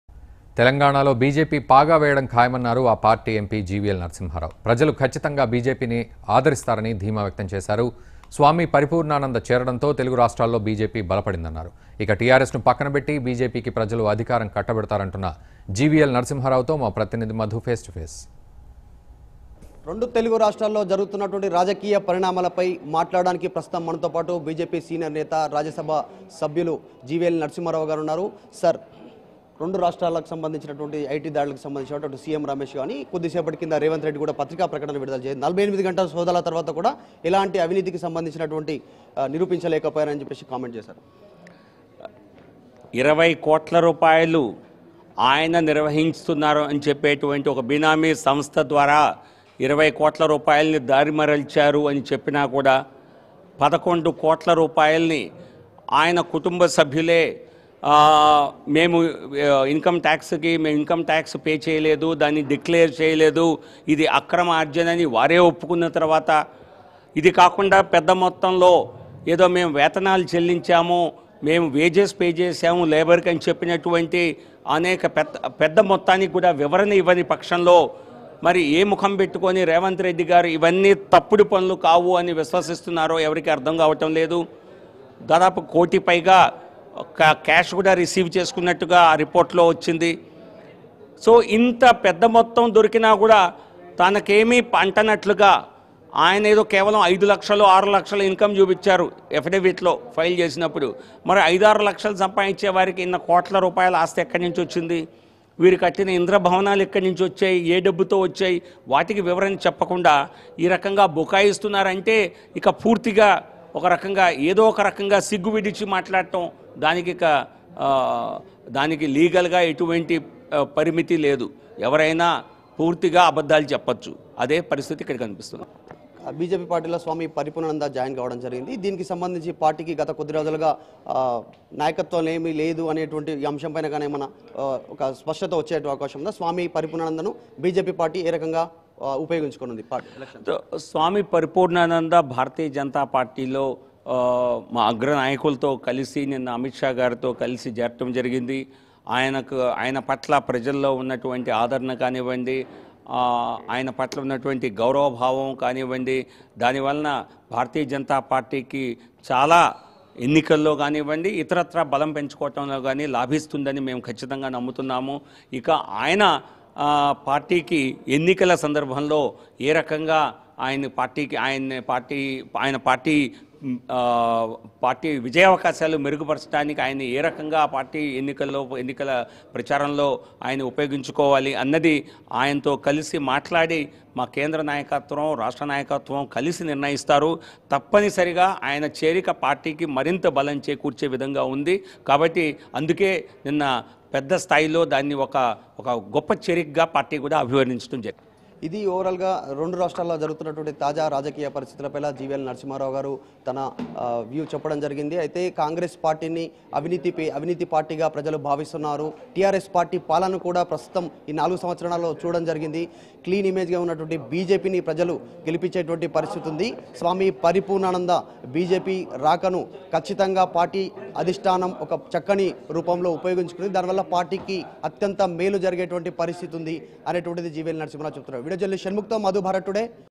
BJP Leader GVL Narasimha Rao Face To Face Interview Over IT Raids In TWo States | BharatToday
BJPLeaderGVLNarasimhaRao Face To Face Interview Over ITRaids In TWo States | BharatToday